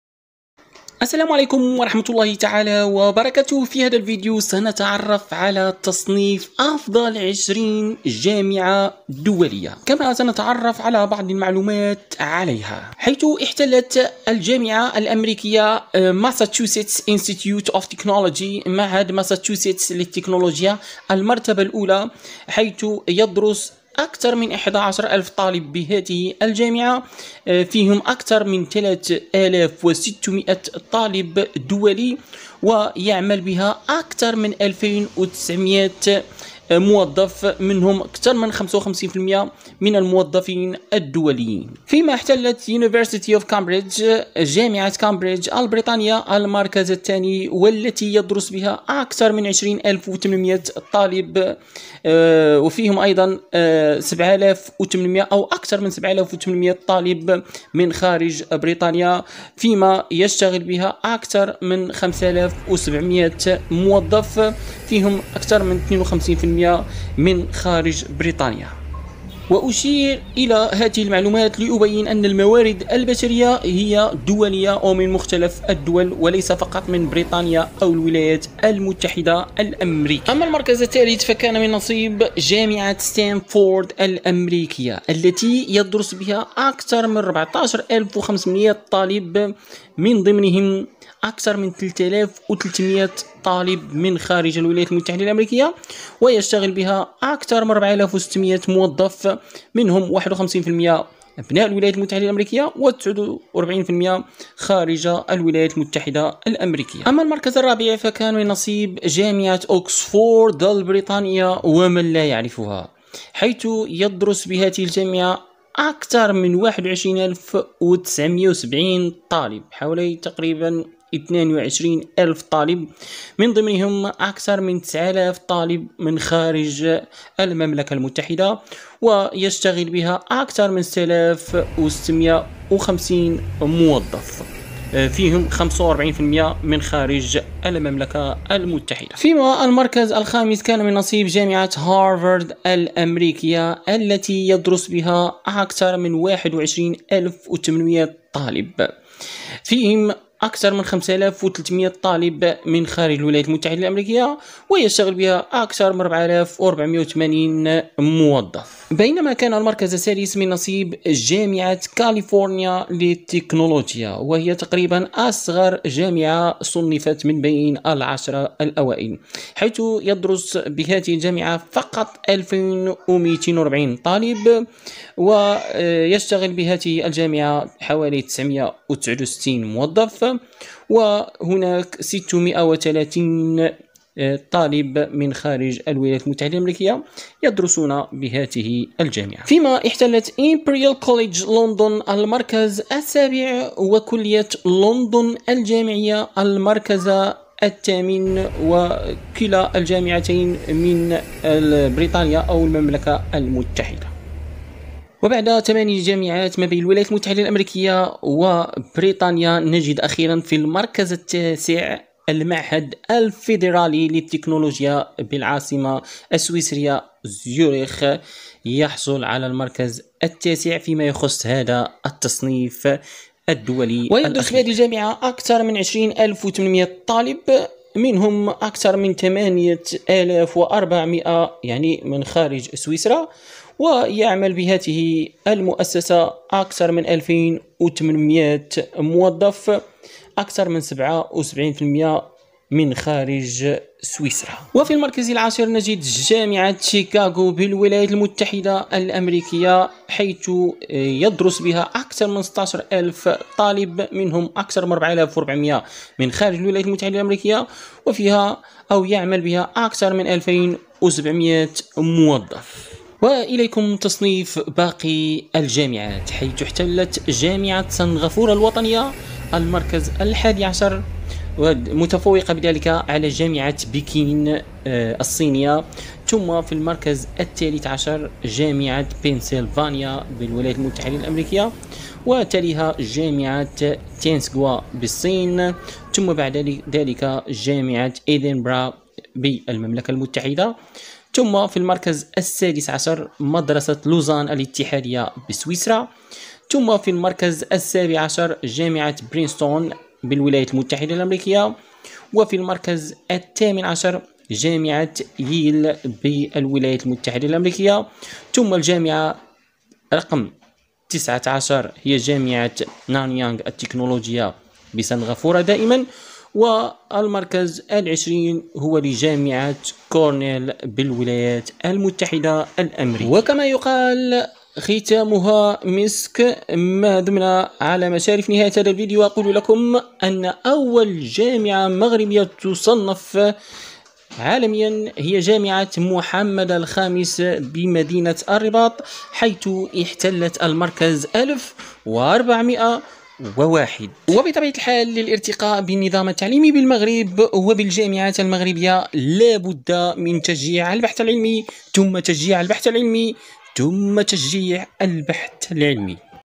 أفضل الجامعات العالمية والمغربية وتصنيفها لسنة 2023
تصنيف أفضل الجامعات العالمية وبعض المعلومات عن عدد طلابها وموظفيها

أمّا الجامعات التي احتلت المرتبة من 11 إلى 25 في التصنيف لعام 2023 هي:
.11

جامعة سنغافورة الوطنية  National University of Singapore  (سنغافورة)

12.

جامعة بكين Peking University (الصين)

13.

جامعة بنسلفانيا University of Pennsylvania (الولايات المتحدة)

14.

جامعة تسنغوا Tsinghua University (الصين)

15.

جامعة ادنبرة  University of Edinburgh (المملكة المتحدة)

16.

مدرسة لوزان الاتحادية EPFL (سويسرا)

17.

جامعة برينستون  Princeton University (الولايات المتحدة الأمريكية)

18.

جامعة ييل      Yale University (الولايات المتحدة الأمريكية)

19.

جامعة نانيانغ التكنولوجية Nanyang Technological University (سنغافورة)

20.

جامعة كورنيل Cornell University (الولايات المتحدة الأمريكية)

21.

جامعة هونج كونج  University of Hong Kong(هونج كونج)

22

جامعة كولومبيا  Columbia University (الولايات المتحدة الأمريكية)

23.

جامعة طوكيو University of Tokyo (اليابان)

24.

جامعة جون هوبكنز  John Hopkins University(الولايات المتحدة الأمريكية)

25.

جامعة ميشيغان آن أربور University of Michigan Ann-Arbor (الولايات المتحدة الأمريكية)

ماذا عن أفضل 50 جامعة في العالم في تصنيف كيو إس للعام 2023؟
اتسع تصنيف أفضل 25 جامعة ليشمل العديد من الجامعات من وجهات دراسية أخرى منها أستراليا وكندا بجانب الولايات المتحدة الأمريكية والمملكة المتحدة، مما يوضح الأداء القوي للجامعات في هذه الوجهات. حيث تفتخر الولايات المتحدة الأمريكية بوجود 13 جامعة من بين أفضل 50 جامعة، أمّا المملكة المتحدة تنافس بثماني جامعات، وتشارك أستراليا بخمس جامعات، وكندا بثلاثة جامعات.